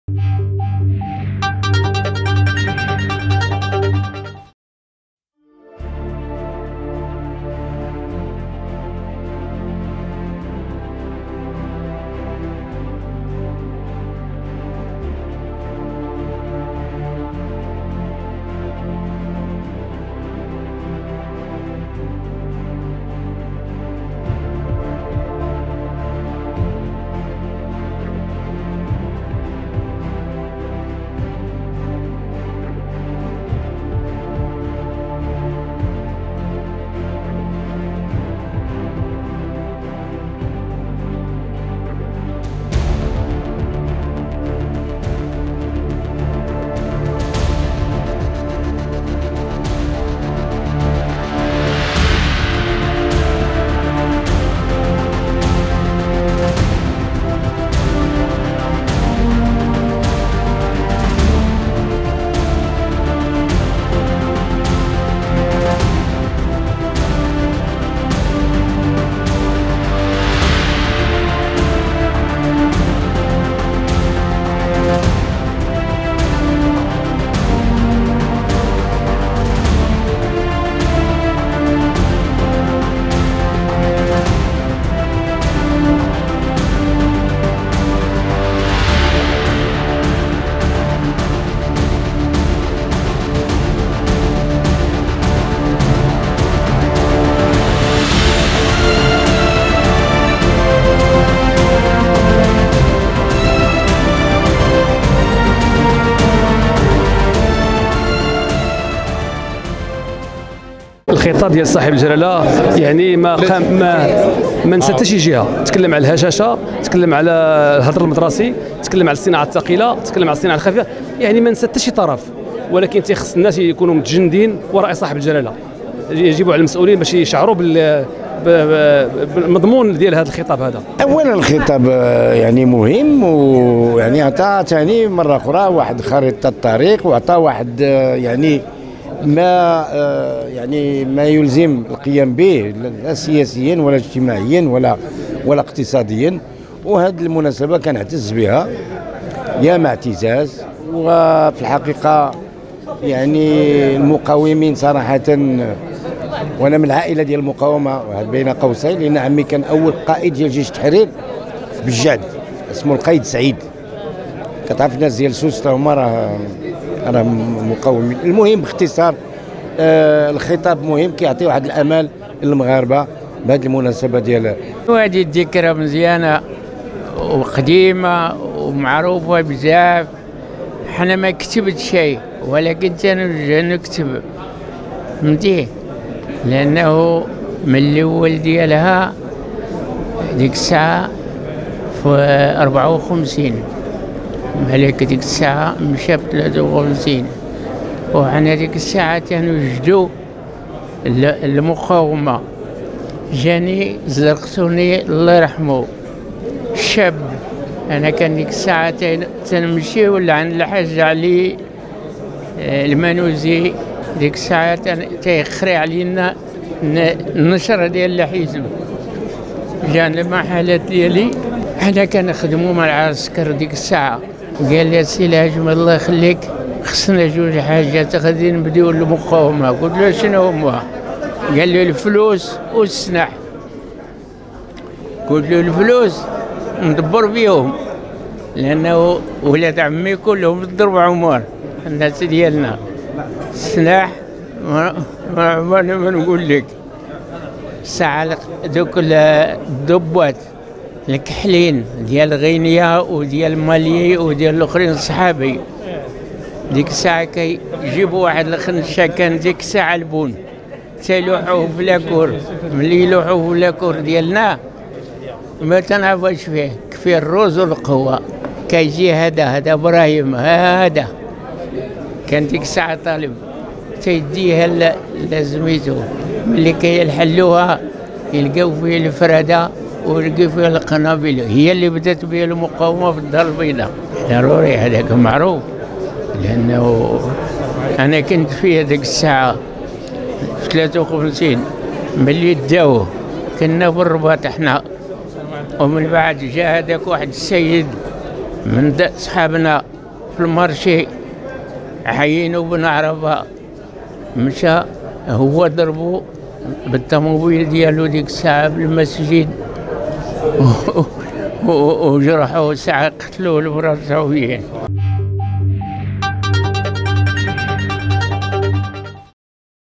هكذا تابع الفكاهي بنياز خطاب ثورة الملك و الشعب إلى جانب المقاومين بولاية الدارالبيضاء
وضع الخطاب الذي وجهه الملك محمد السادس للأمة بمناسبة الذكرى السادسة والستين لثورة الملك والشعب
"التصور العام" لعمل اللجنة الخاصة بالنموذج التنموي الجديد للمملكة